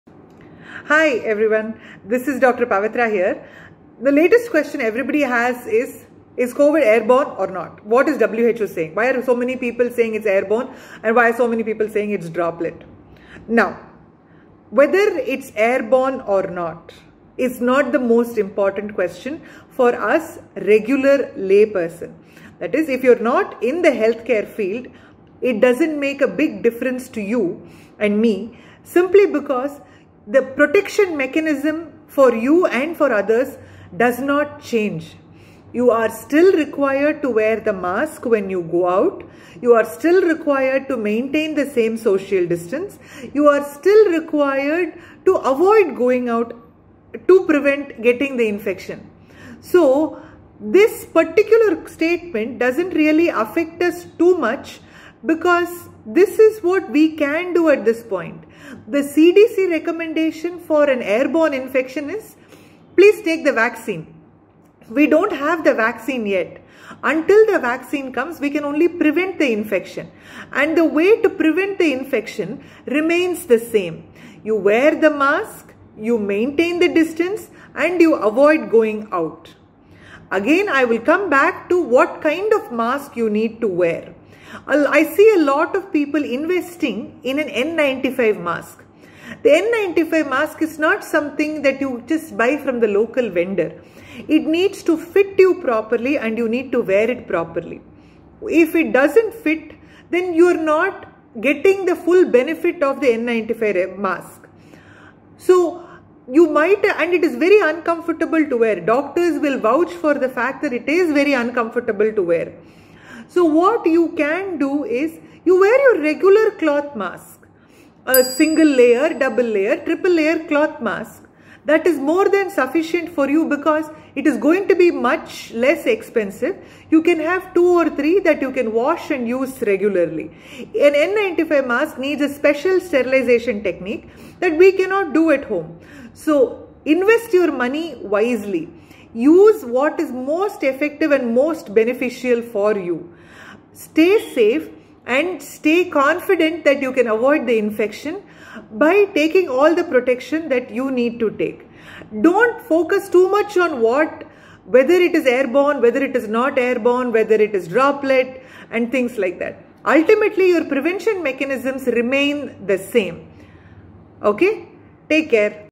Is COVID19 airborne?
Can Coronavirus be transmitted through air? Does that mean a greater risk of infection? What will we do to avoid it?!
These are just some of the questions that Dr. Pavithra Venkatagopalan answers in this video today.